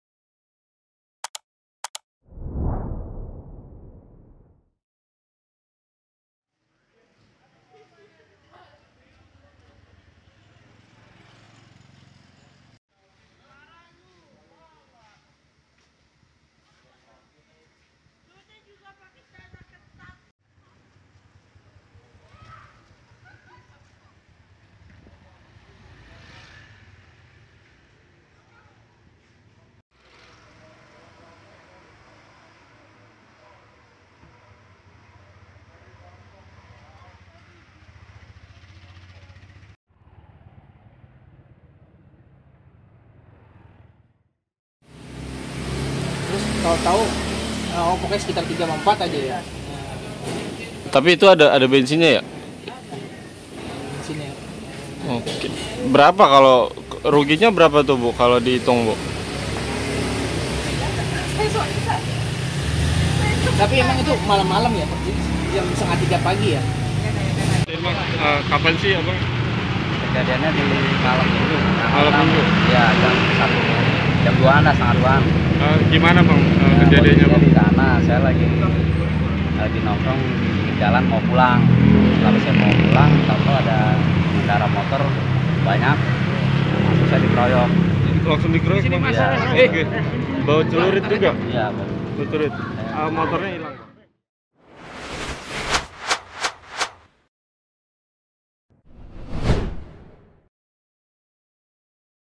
TANGERANG SELATAN, SEKELOMPOK ORANG NGAMUK SAMBIL BAWA SAJAM, 4 BOTOL BENSIN ECERAN DICURI
TANGERANG SELATAN, Sekelompok orang tidak dikenal yang mengamuk di wilayah Kecamatan Pondok Aren, Kota Tangerang Selatan (Tangsel), bukan hanya merusak tempat cuci tangan masjid setempat. Namun, mereka juga berbuat aksi beringas dengan mencuri bensin botol dari pedagang eceran. Tak tanggung-tanggung, empat botol bensin dicurinya.

Pedagang bensin eceran tersebut bernama Dewi Amor (30). Dewi mengaku, empat botol bensin miliknya dicuri oleh sekolompok orang yang mengamuk tersebut.

"Ada empat botol mas yang hilang sama botol-botolnya. Rak tempat botol bensin memang tidak digembok, cuma ditutup saja. Jadi mereka bisa ambil begitu saja," ujarnya kepada Poskota di lokasi, Senin (8/3/2021).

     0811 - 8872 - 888